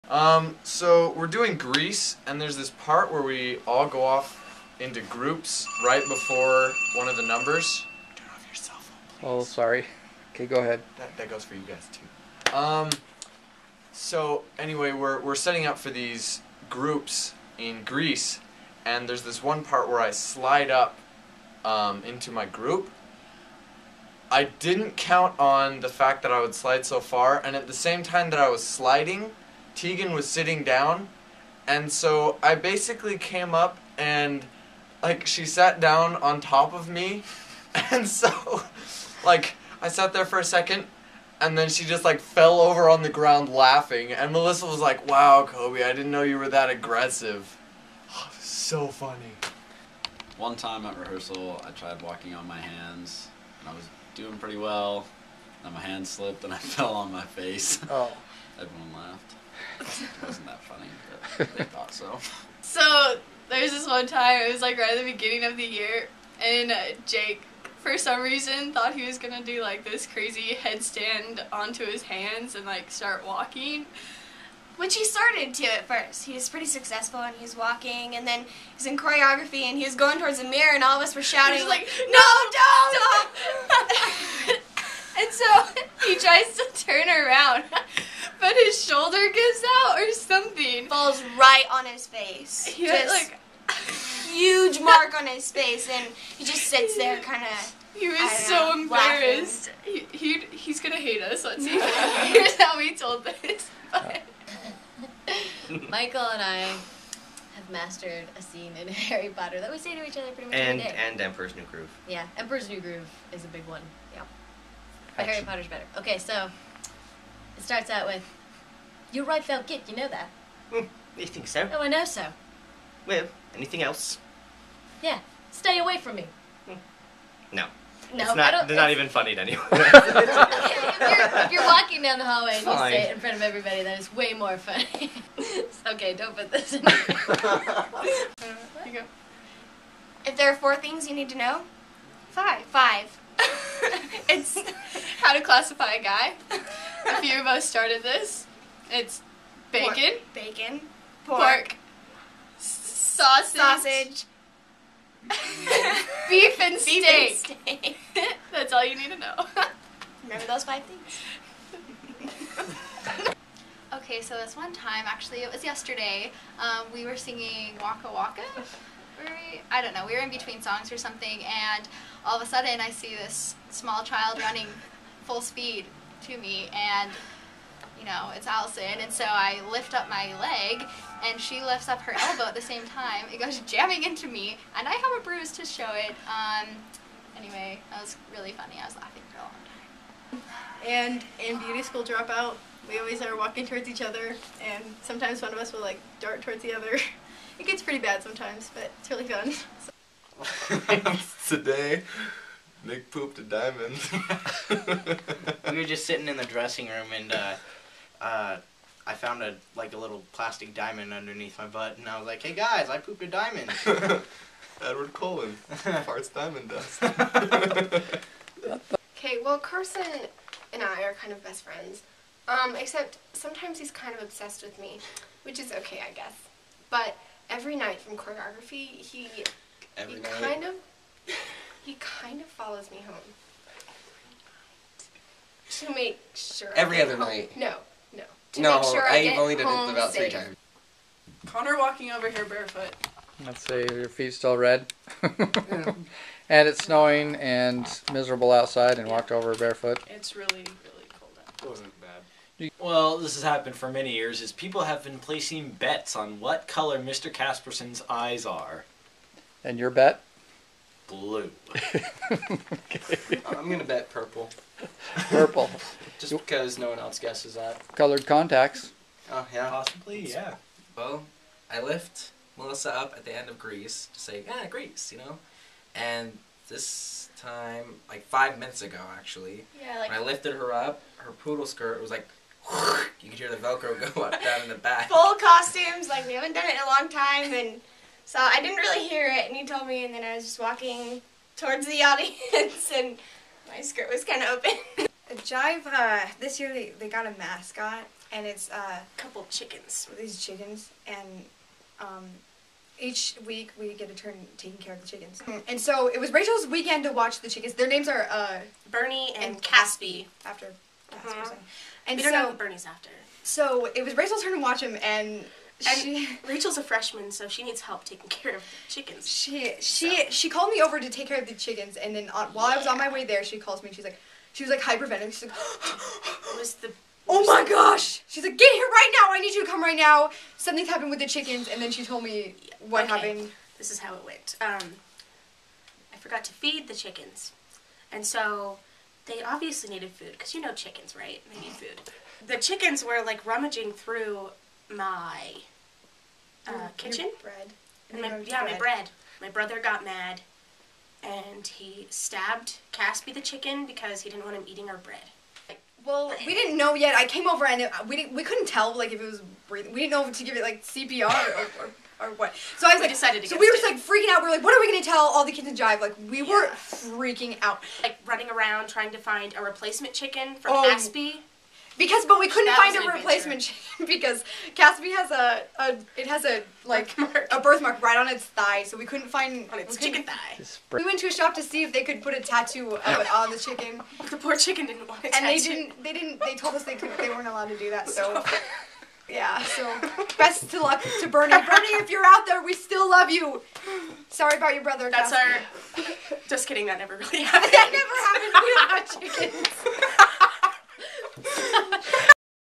Jive 2012 members telling funny stories
Jive 2012 members telling funny stories about things that happened in Jive.